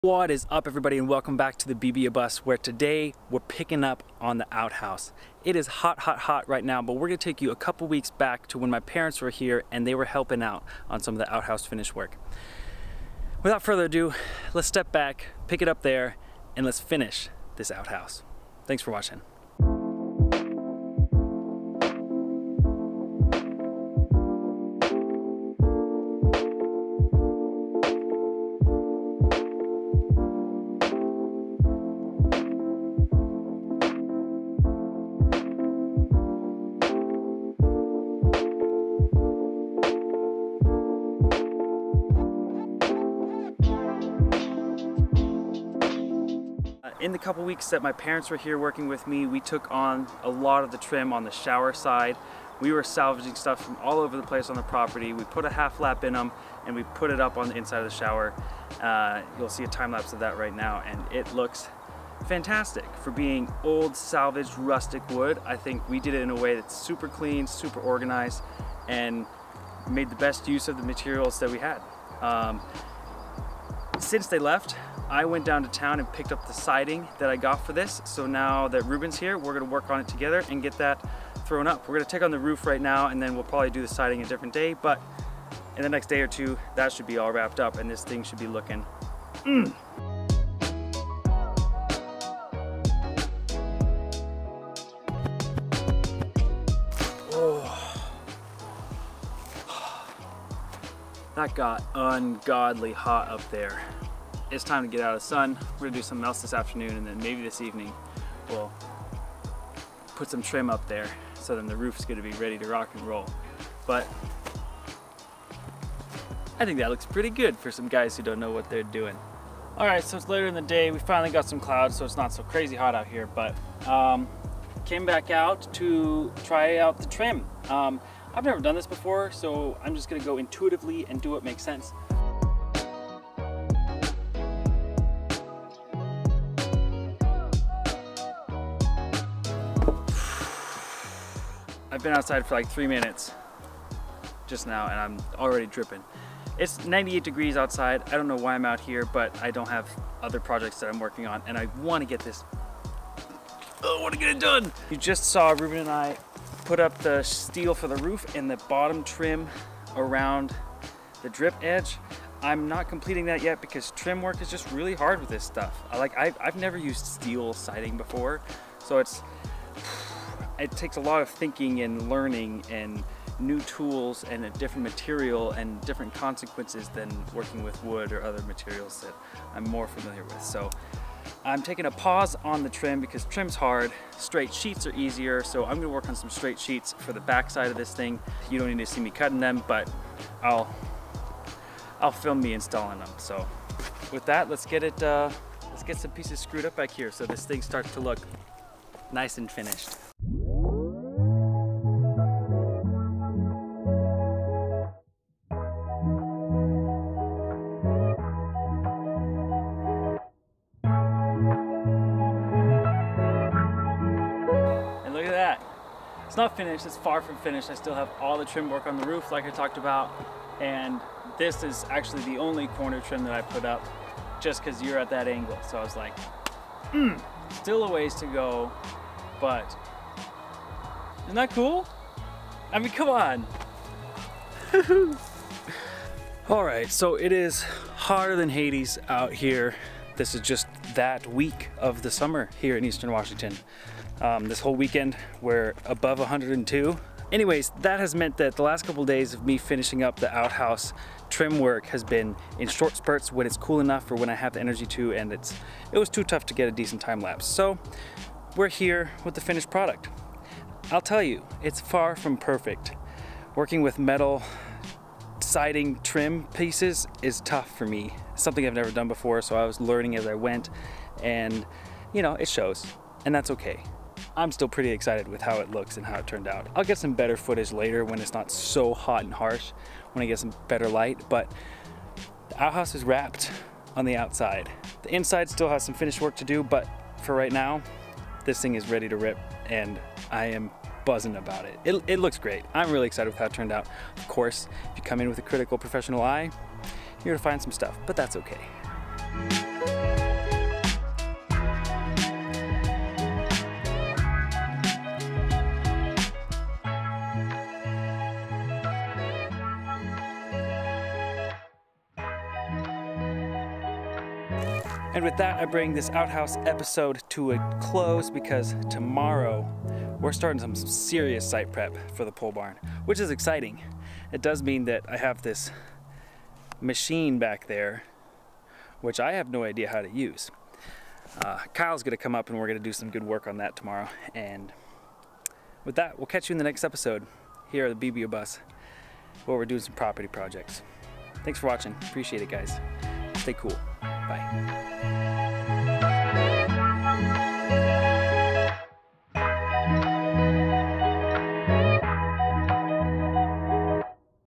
Finishing the Outhouse (Exterior)
What do you think?! So so so pumped to see how this turned out! Huge learning process but confident with the outcome.

Should I sell the plans for this?